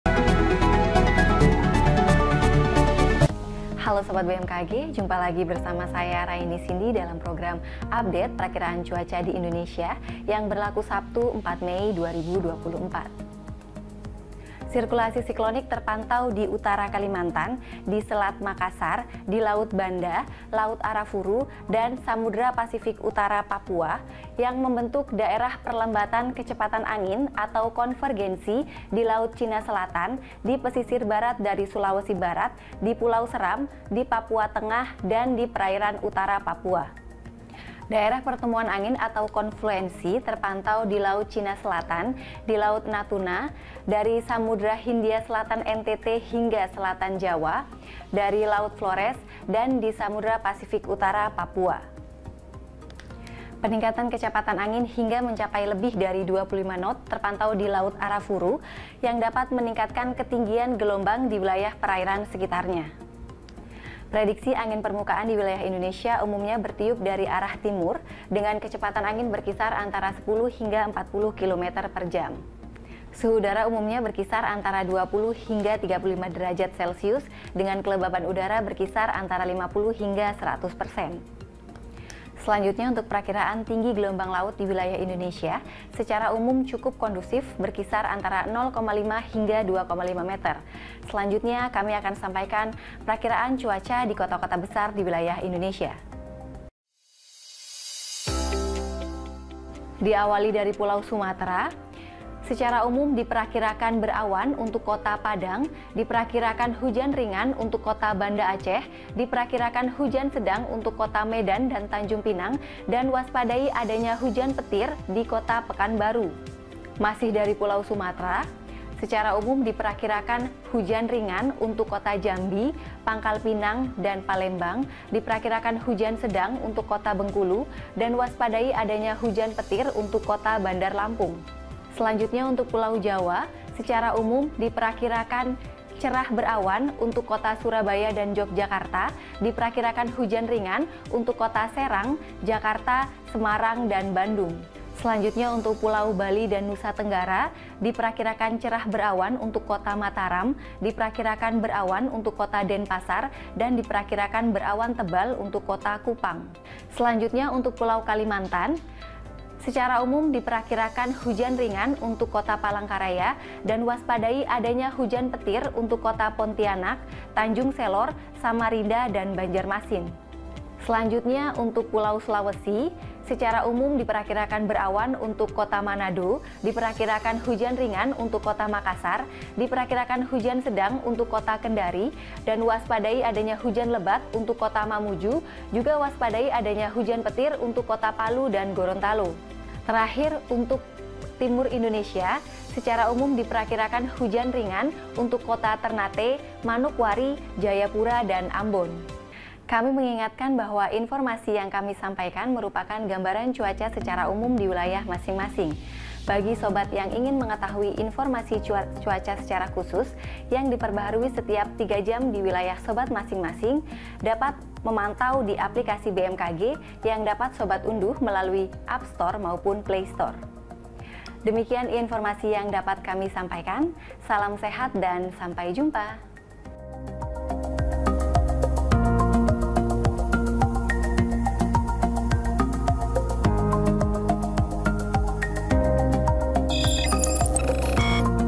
Prakiraan Cuaca Esok Hari Sabtu, 04 Mei 2024
Berikut disampaikan tayangan update Prakiraan Cuaca Esok Hari, Sabtu, 04 Mei 2024.